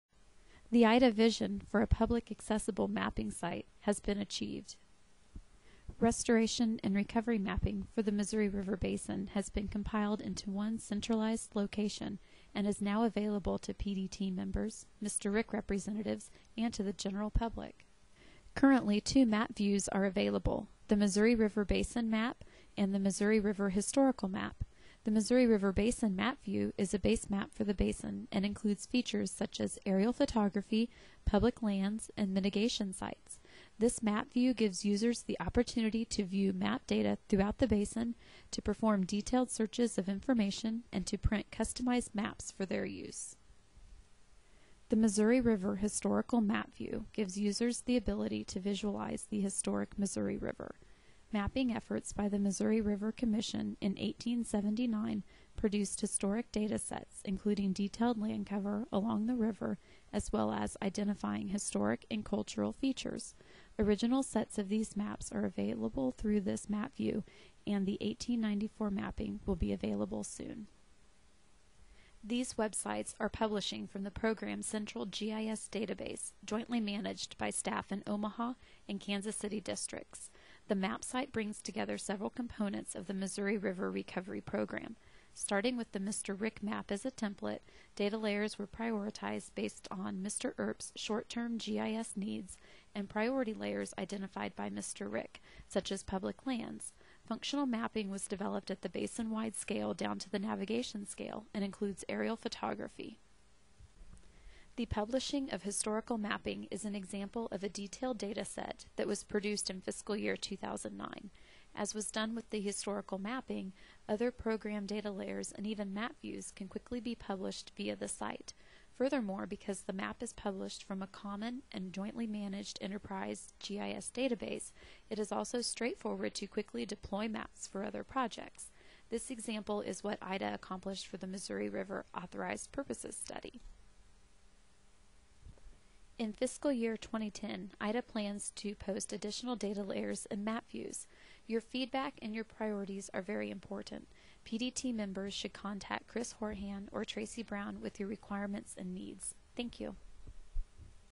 Overview of the Missouri River Recovery Program (MRRP) Mapping Application
The Missouri River Recovery Program's (MRRP) mission is to implement actions to accomplish Missouri River ecosystem recovery goals in coordination and collaboration with agency partners and stakeholders.   The MRRP released its Public Mapping Application in the fall of 2009. The mapping site will be used by all elements of the MRRP to make geospatial information available to the public. You can also find the mapping application through program's official web

This video provides a brief overview of the mapping site and a discussion of the planned additions to enhance its capabilities